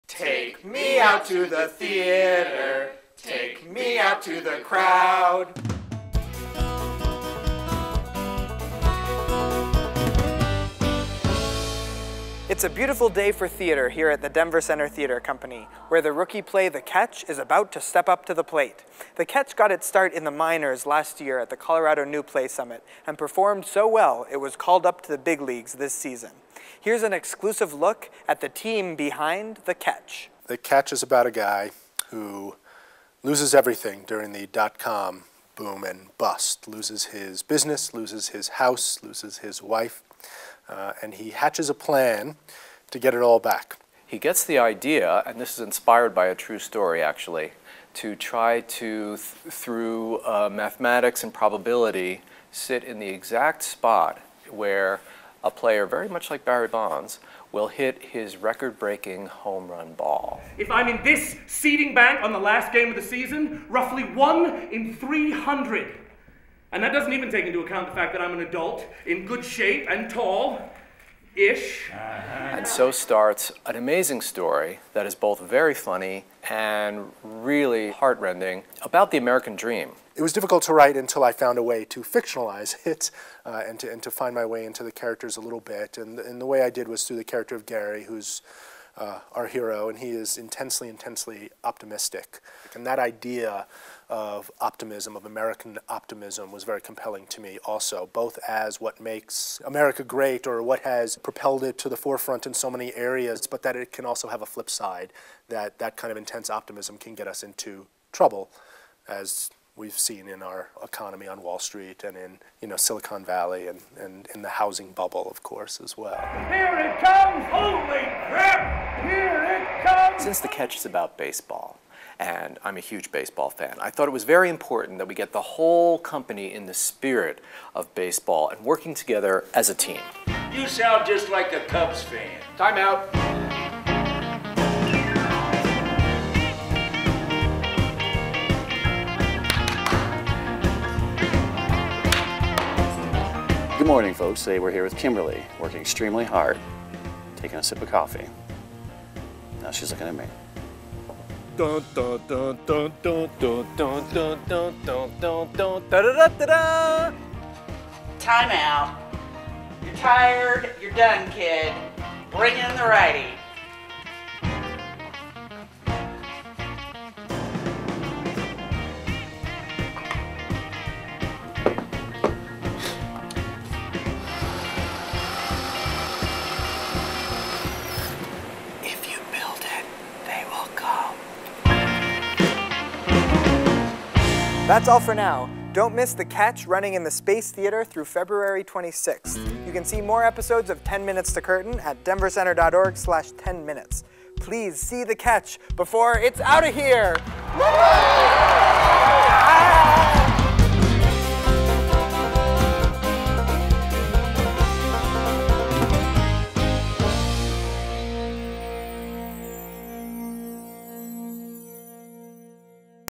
THE CATCH - 10 Minutes to Curtain
Go behind-the-scenes and see how the Denver Center Theatre Company is preparing for the world premiere production of THE CATCH by Ken Weitzman, a play about baseball, riches, and celebrity.